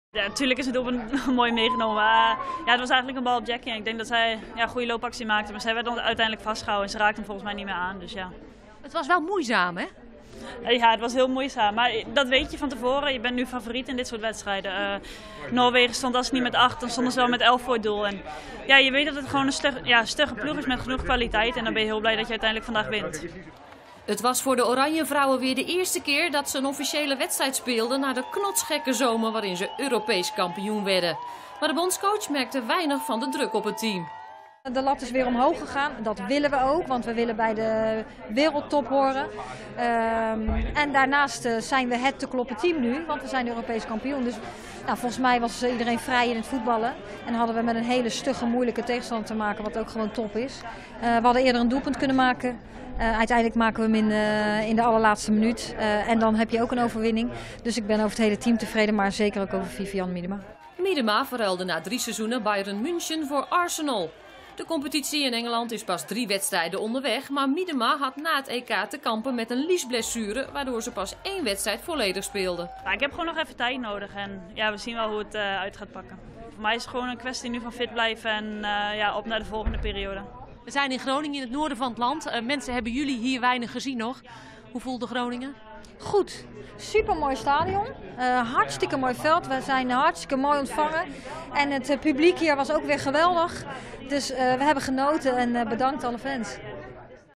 Vivianne Miedema matchwinner in Groningen tegen Noorwegen
De Oranjevoetbalsters hebben gisteren de eerste stap richting het wereldkampioenschap van 2019 in Frankrijk gezet. In blessuretijd schoot Vivianne Miedema uit Hoogeveen de ploeg naar de 1-0 overwinning op Noorwegen.